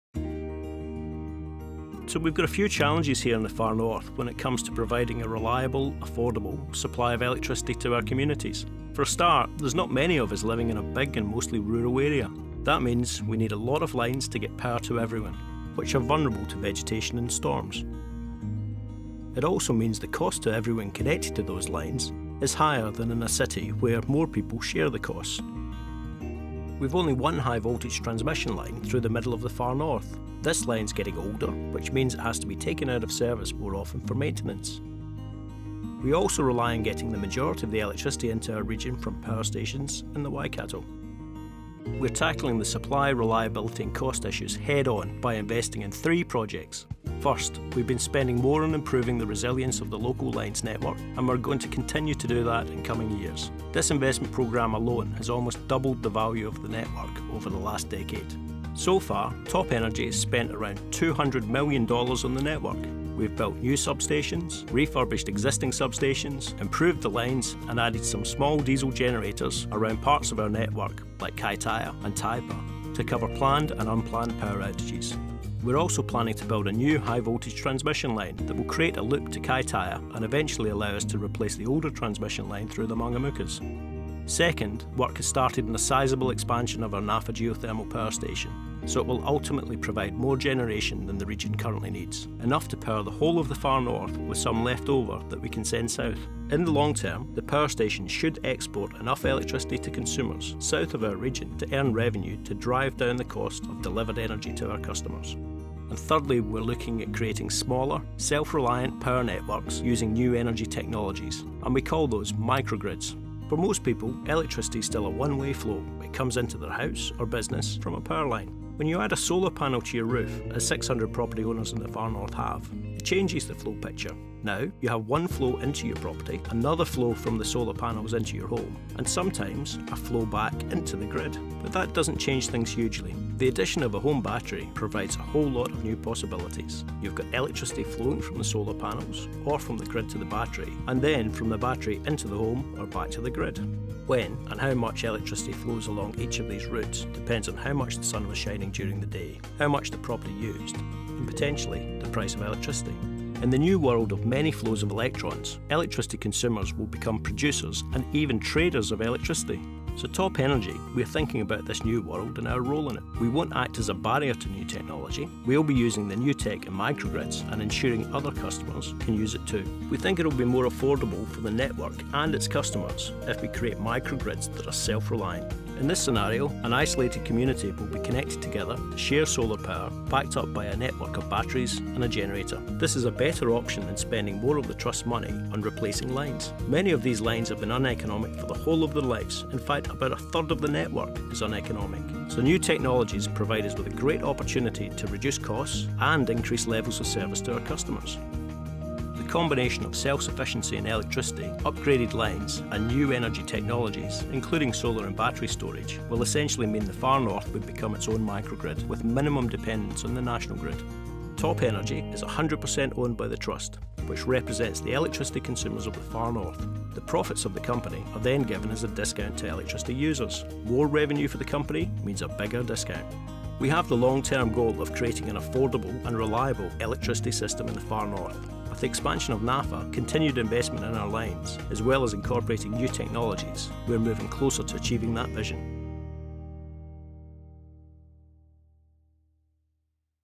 Top Energy - Energy of the Future
Far North electricty distribution and geothermal generation company Top Energy explains its long term energy strategy and network management